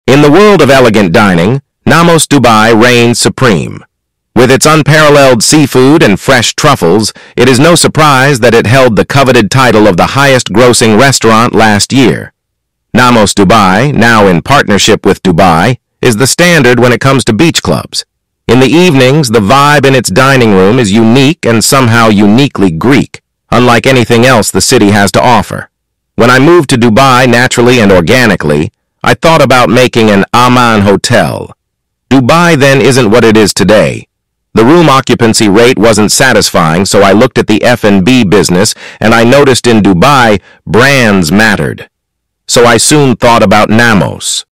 Exploring Dubai’s Rooftop Dining Scene - What Leaders Need To Know
In the world of elegant dining, Nammos Dubai reigns supreme. With its unparalleled seafood and fresh truffles, it is no surprise that it held the coveted title of the highest-grossing restaurant last year. In the evenings, the vibe in its dining room is unique and somehow uniquely Greek – unlike anything else the city has to offer.

"When I moved to Dubai naturally and organically, I thought about making an AMAN Hotel. Dubai then isn’t what it is today. The room occupancy rate wasn’t satisfying, so I looked at the F&B business and I noticed in Dubai, brands mattered. So I soon thought about NAMMOS."